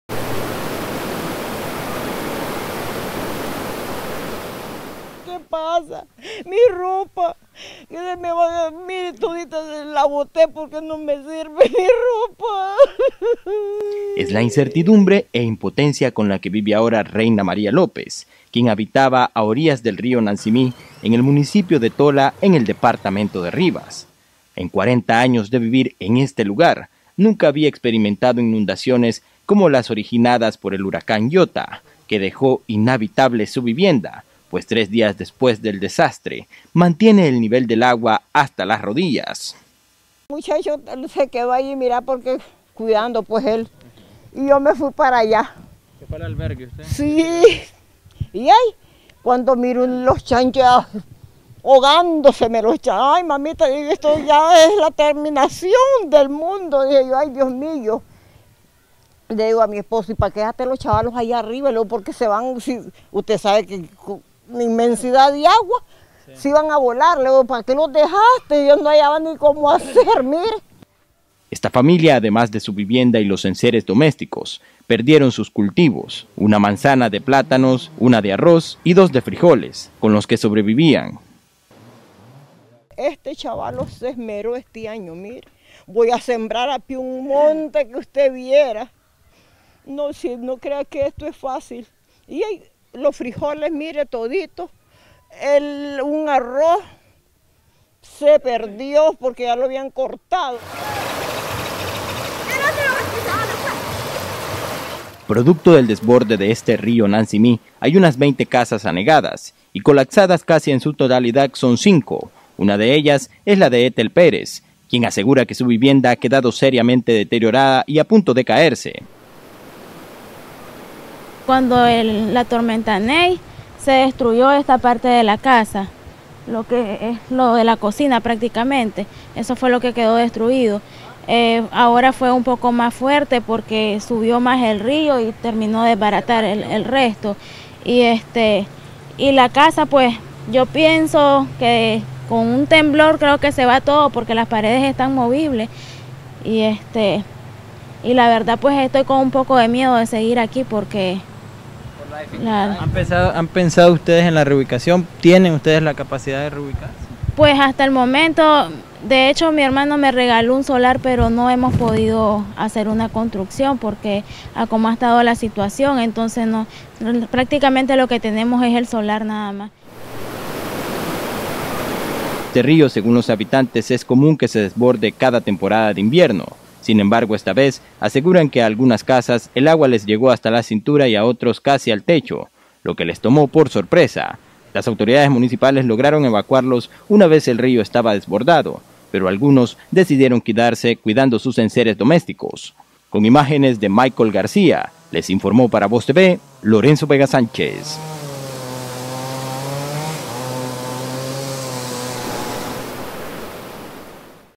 REPORTAJE ESPECIAL: Estragos que dejó el huracán Iota en Tola
En un escenario de tristeza y destrucción se ha convertido la comunidad indígena de Nacimí en Tola.

Tejas, maderas y escombros dominan el panorama de una de las localidades más afectadas por el paso de Iota al sur de Nicaragua.